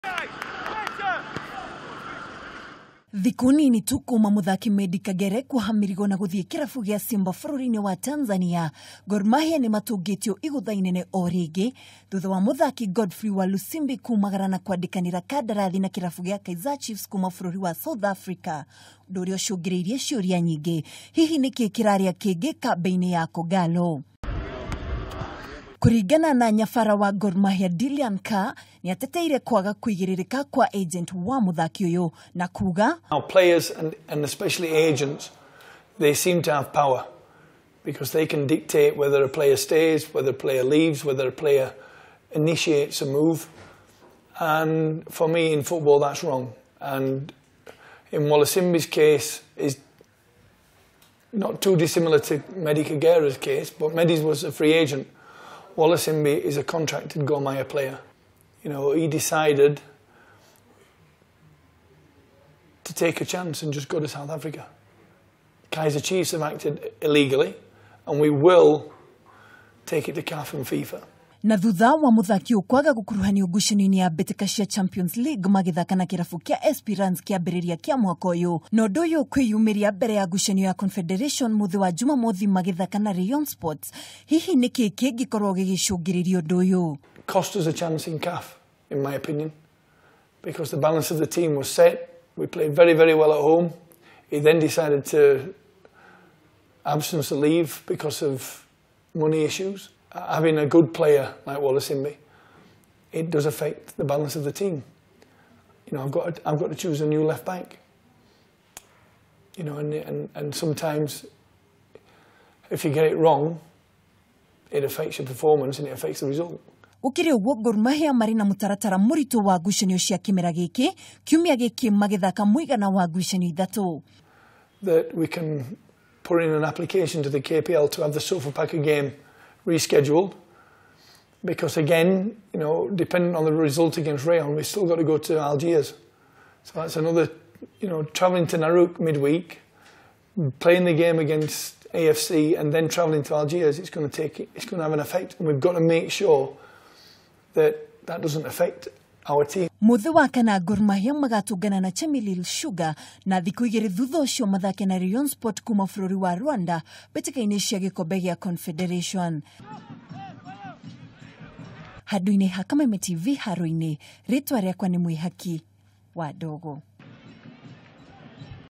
Godfrey Walusimbi kumagara na njira iri na kiriga
Mweri umwe tu thutha wa muthaki wa kirabu kia Gor Mahia Meddie Kagere kumagara na guthii kirabu gia Simba bururi-ini wa Tanzania, Gor Mahia oo ringi nimatungitio igutha inene thutah wa muthaki Godfreey Walusimbi kumagara atari na rutha niguo gwikanirira kandarathi na kirabu gia Kaizer Chiefs bururi-ini wa South Africa undu uria utumurite ciuria nyingi.